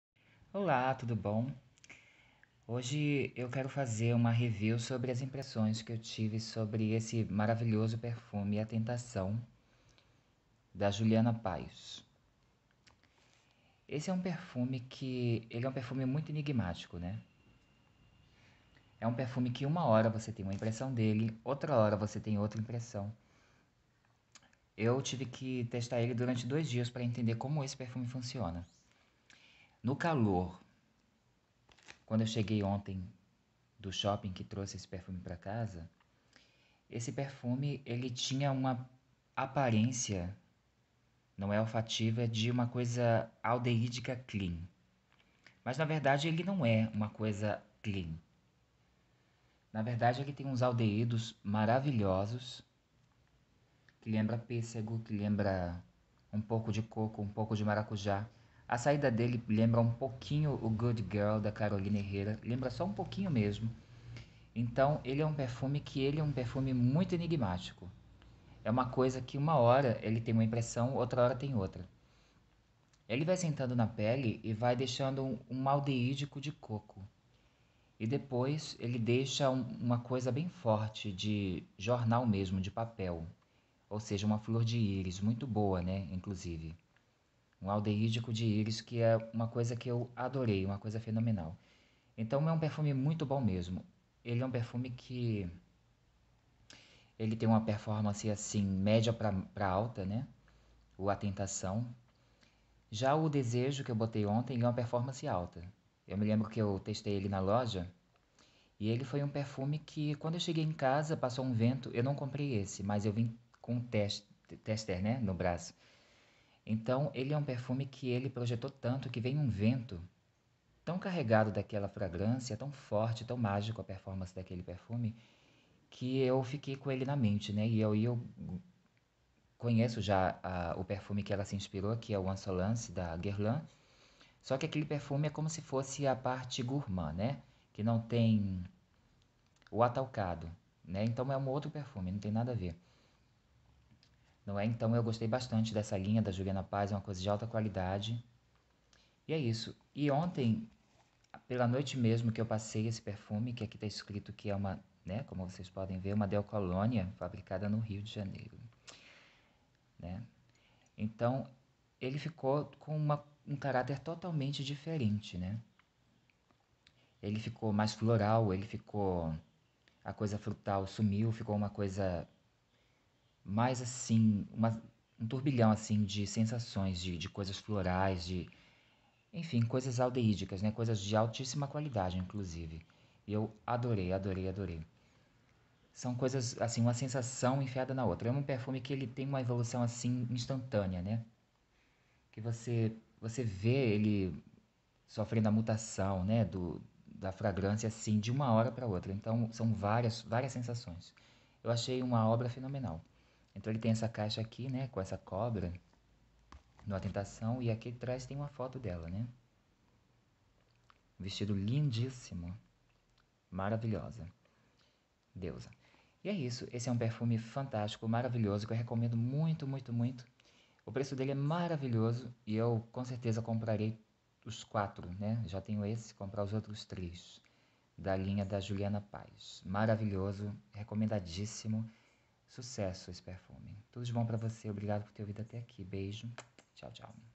Perfume: Juliana Paes - A Tentação
Excelente floral aldeídico. Um encanto.